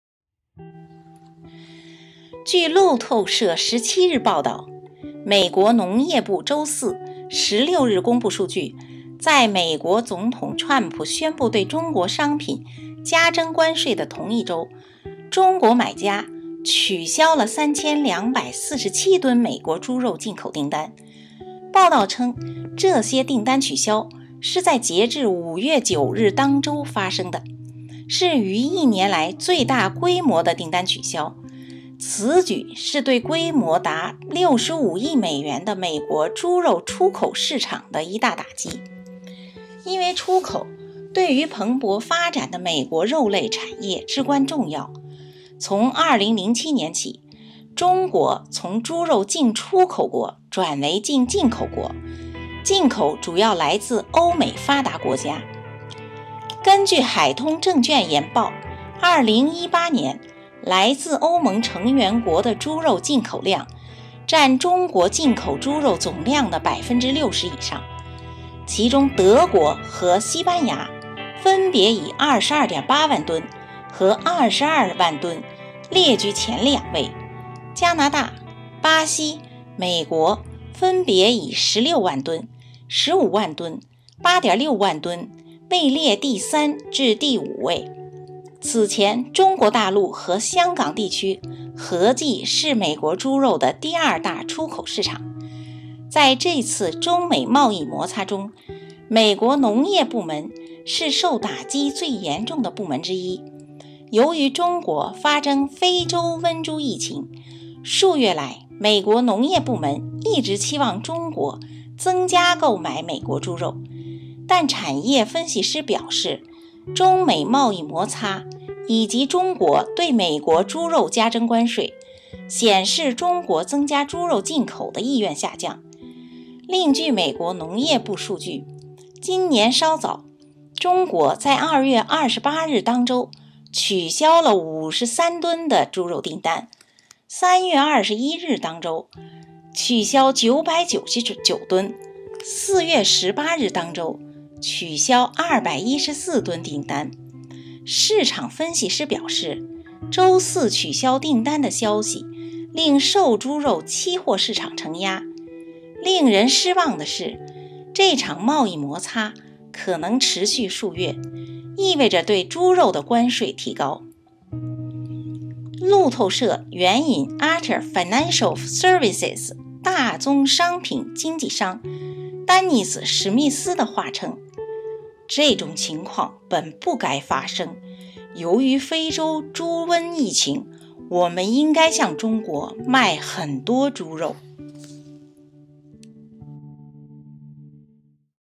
中国取消3247吨美国猪肉订单 这多猪，咋整？
美国人不爱吃猪肉  中国人没猪肉不吃饭
取消美国猪肉订单，这多猪，咋整？
政府补贴养猪户，也补不了那份辛苦与伤心 谁买偶的猪？